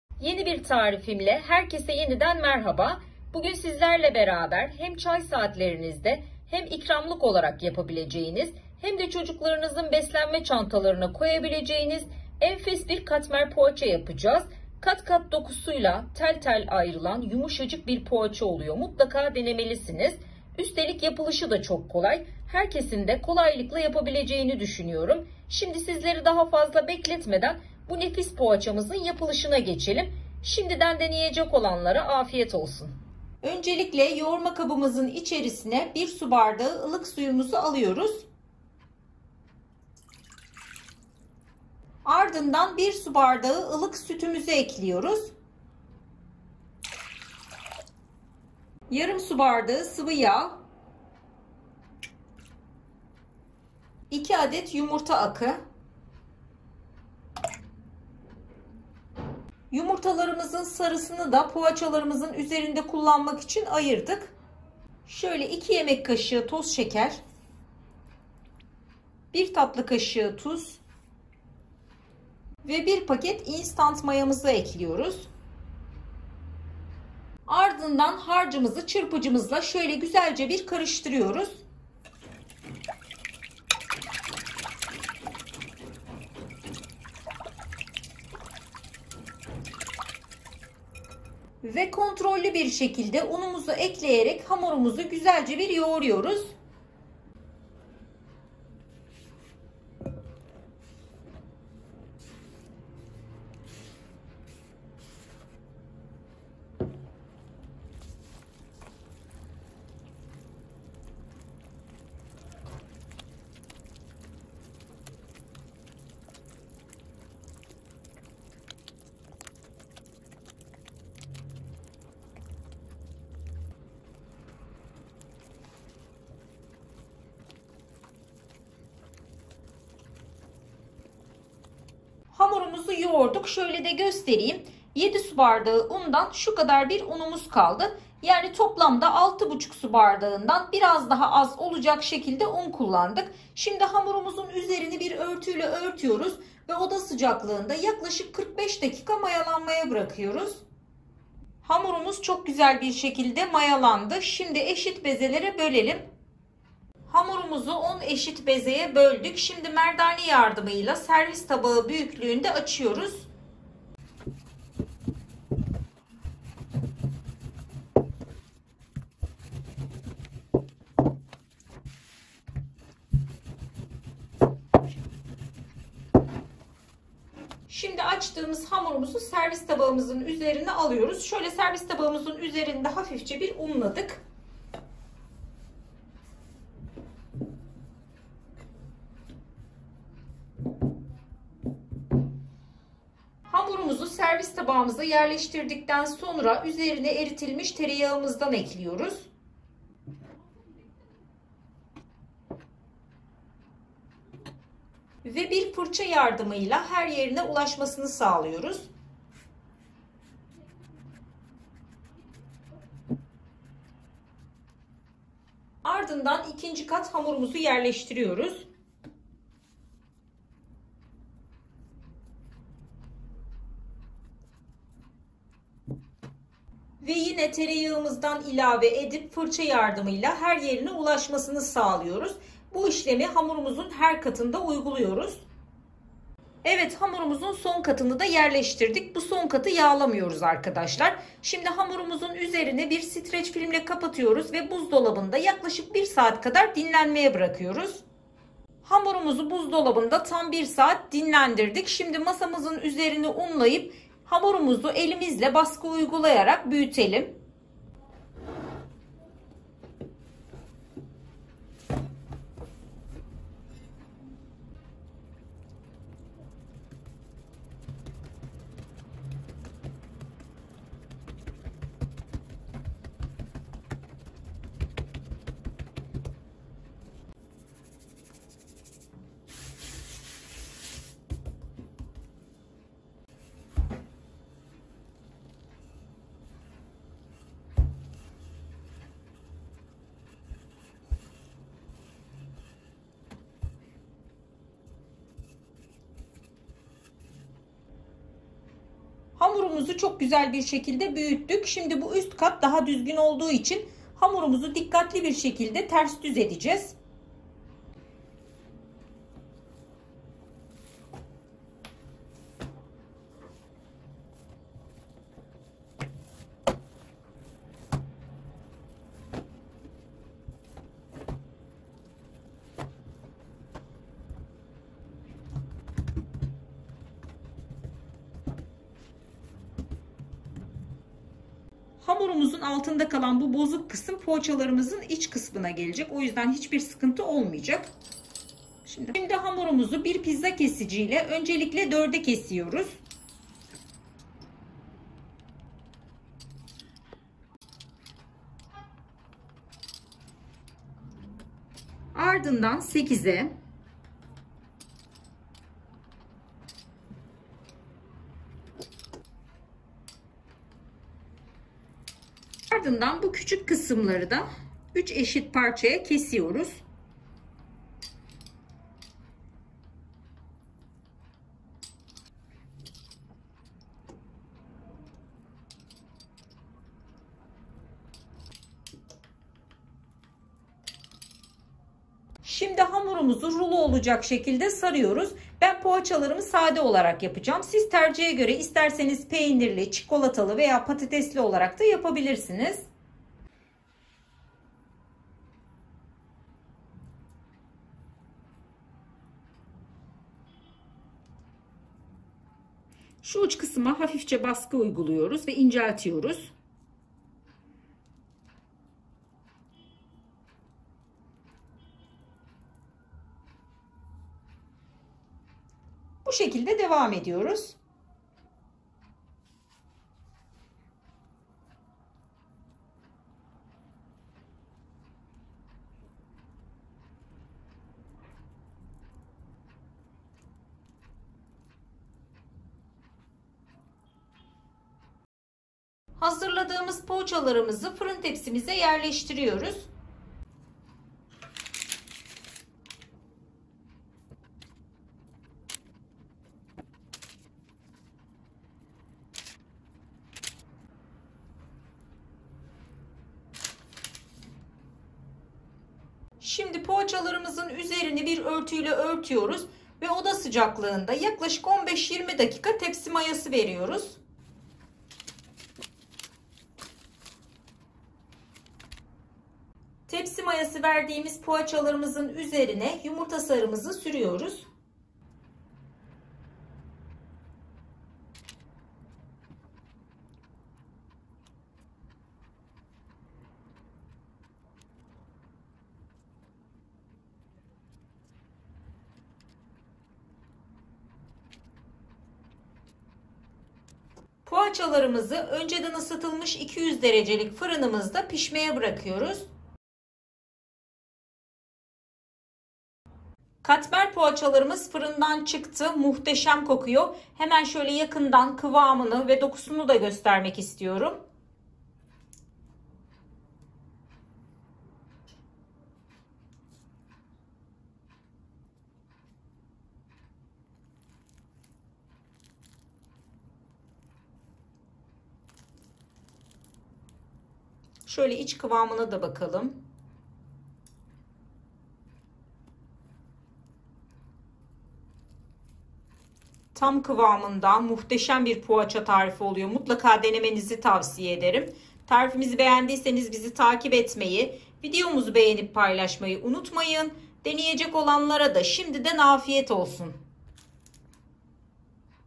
TADINA DOYAMAYACAKSINIZ ✌️ Favoriniz Olacak Katmer Poğaça Tarifi / Poğaça tarifleri
Sabahın erken saatlerinde mis gibi kokular eşliğinde gözlerinizi açtığınızda, fırından yeni çıkmış bir poğaçanın sıcaklığı ve tereyağının erimiş haliyle buluşan o eşsiz lezzeti aklınıza gelir mi? Her ısırıkta damağınızda dağılan yumuşacık hamuru ve içindeki sürpriz malzemeleriyle poğaça, sadece bir kahvaltı klasiği değil, aynı zamanda günün her anında size keyifli bir kaçamak sunar. İster peynirli, ister zeytinli, ister patatesli olsun; poğaça, her yaştan insanı kendine çeken bir cazibeye sahiptir. Bu altın rengindeki mucize, geleneksel tariflerin modern dokunuşlarla birleştiği bir lezzet şölenine davet ediyor sizleri. Hazırsanız, bu büyülü Katmer Poğaça  yolculuğuna çıkalım ve Katmer poğaçanın hikayesini beraber keşfedelim! Şimdiden afiyet olsun.

Su bardağı ölçüm 200ml

Katmer Poğaça için Malzemeler,
1 su bardağı ılık su,
1 su bardağı ılık süt,
Yarım su bardağı sıvı yağ,
2 adet yumurta akı,
2 yemek kaşığı toz şeker,
1 tatlı kaşığı tuz,
1 paket instant maya,
6,5 su bardağından biraz daha az Beyaz un
2 adet yumurta sarısı
150 200 gr tereyağı

Water glass measuring 200ml

Ingredients for Katmer Pastry,
1 glass of warm water,
1 glass of warm milk,
Half a glass of oil,
2 egg whites,
2 tablespoons of granulated sugar,
1 teaspoon of salt,
1 packet of instant yeast,
Slightly less than 6.5 cups white flour
2 egg yolks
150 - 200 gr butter

Ayrıca aşağıdaki Tariflerimi de izlemek isteyebilirsiniz